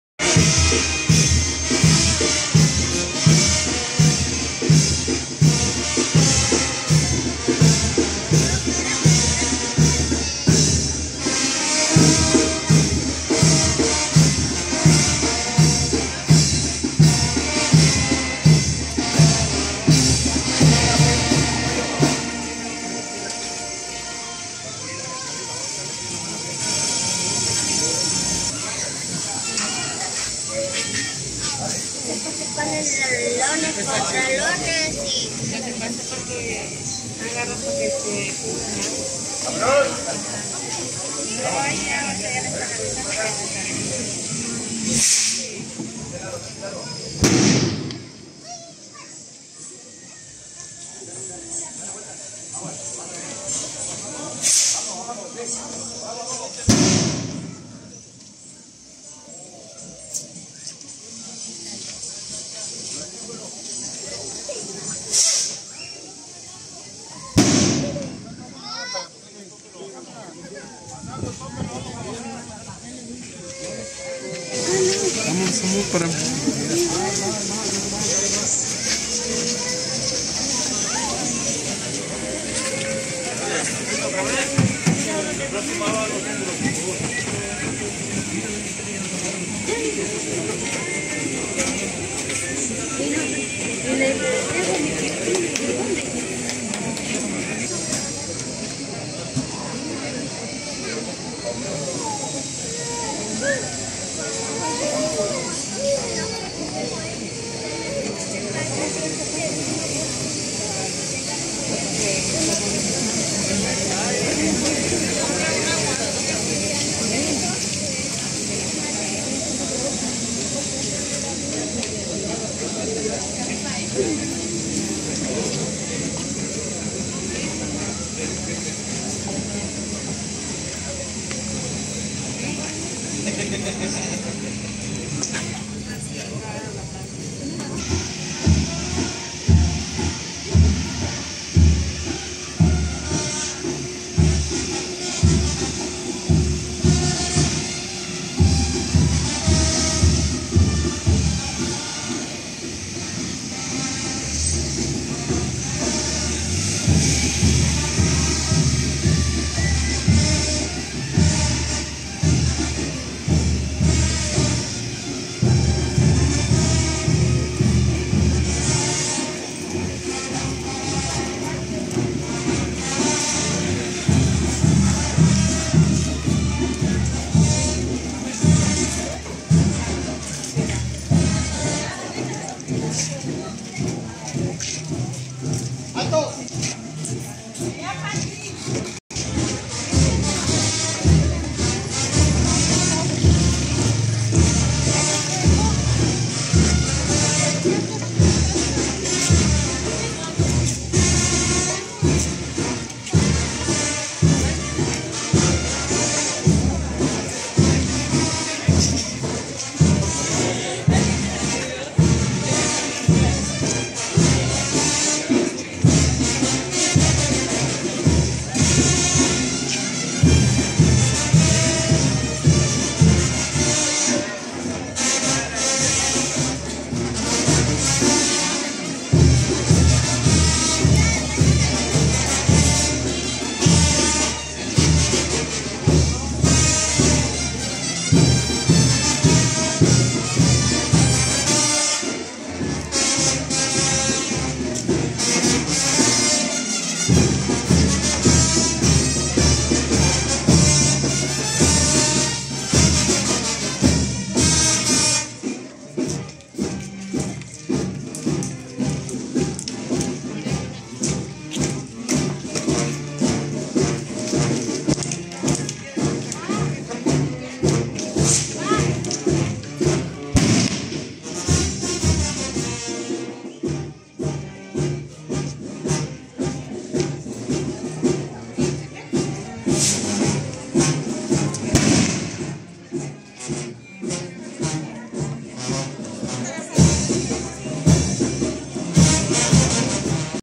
Santiago Apóstol, Jiutepec, Morelos 25 de julio de 2023
Este año no fuimos a Ixcatepec, Guerrero para celebrar al patrón Santiago Apóstol. Pero nos quedamos en Jiutepec, Morelos a ver la profesión. O bueno, lo que se pudo.